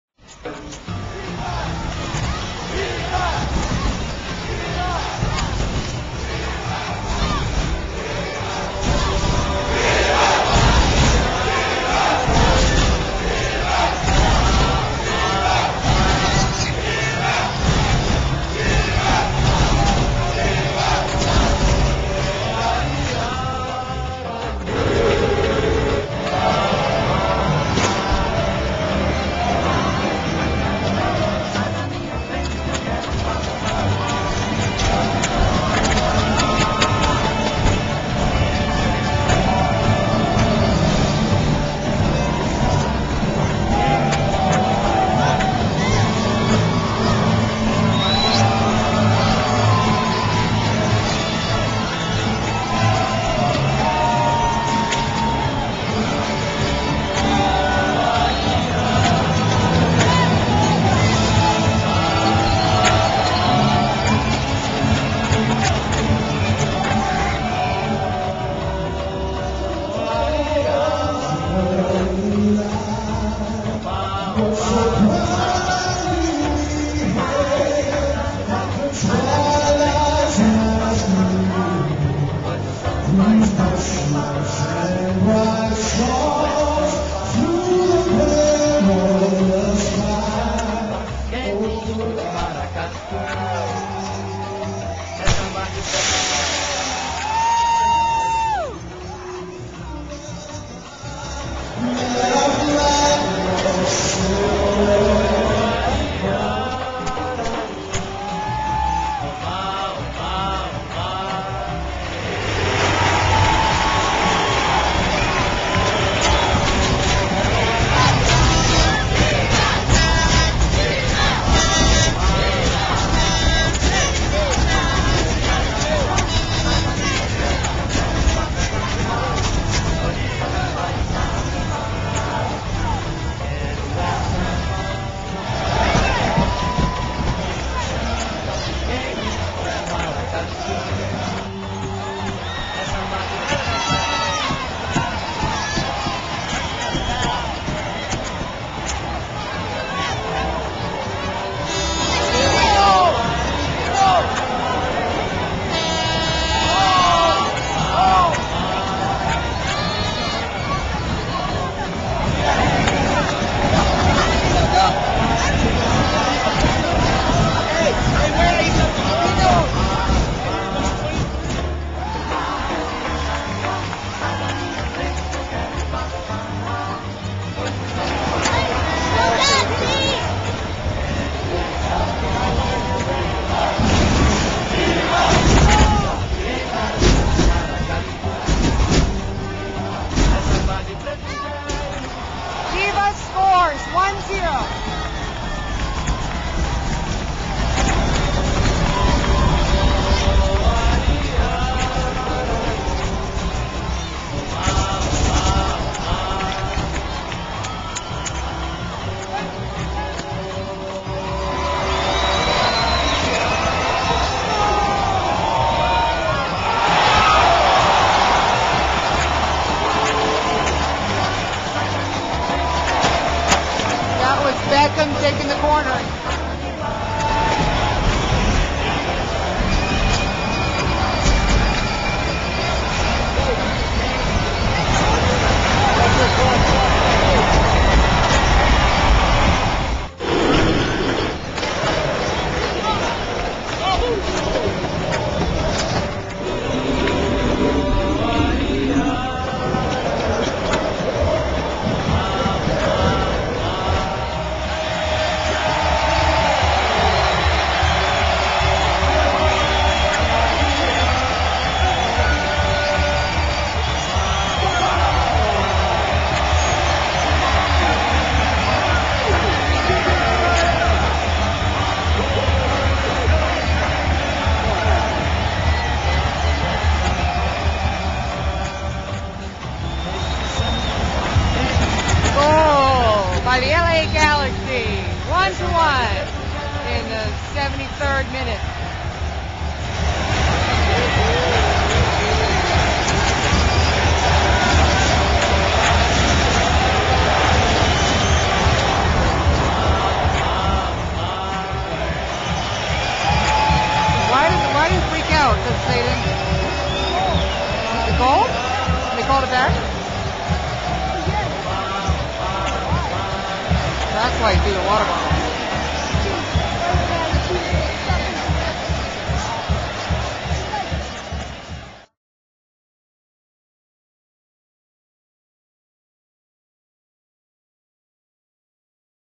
La Galaxy Chivas Part 2
The second part of our trip to the LA game-- loved it!  Exciting time for all of us... Sold out...lots of energy.